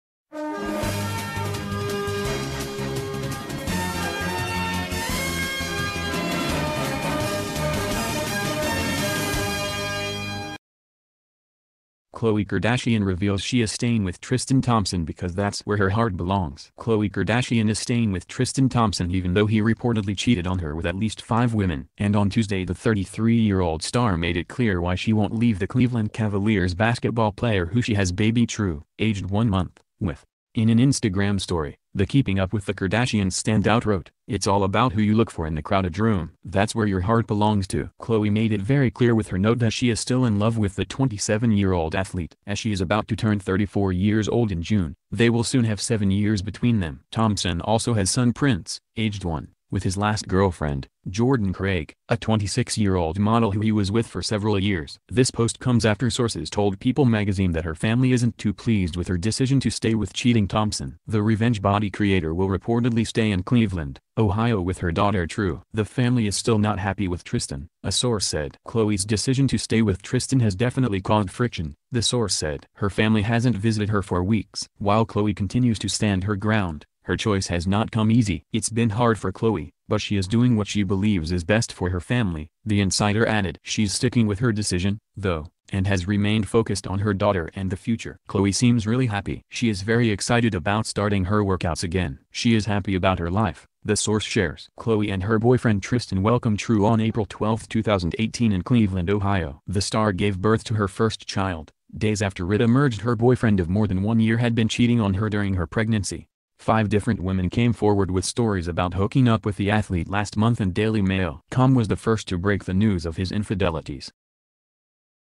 Khloe Kardashian is staying with Tristan Thompson even though he reportedly cheated on her
Khloe Kardashian reveals she is staying with Tristan Thompson because that's where her 'heart belongs'.